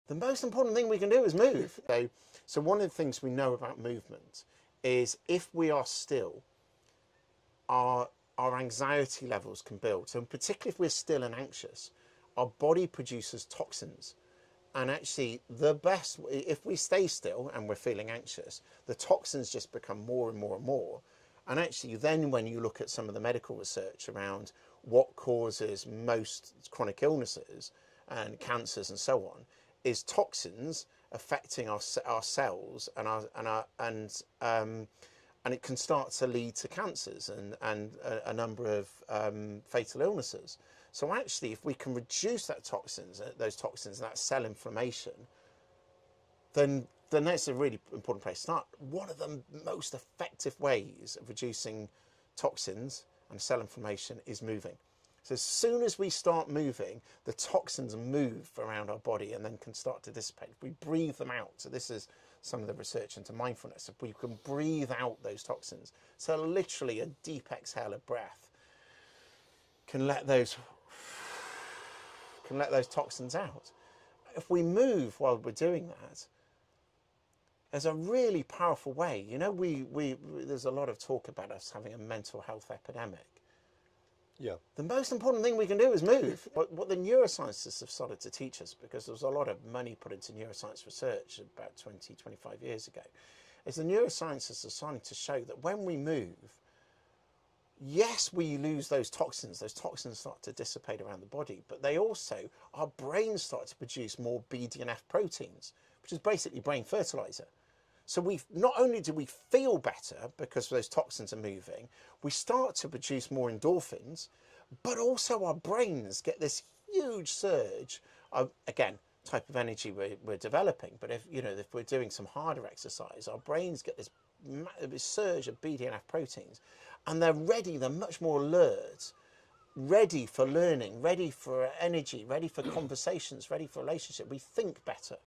Unlock Your Brain: The Power of Movement for Health
Feeling anxious? Staying still can build up toxins. Movement helps dissipate them and boosts BDNF proteins—brain fertilizer. Move to feel better, think better, and be ready for anything! AnxietyRelief ToxinRelease BDNF BrainHealth Movement Mindfulness @mortawel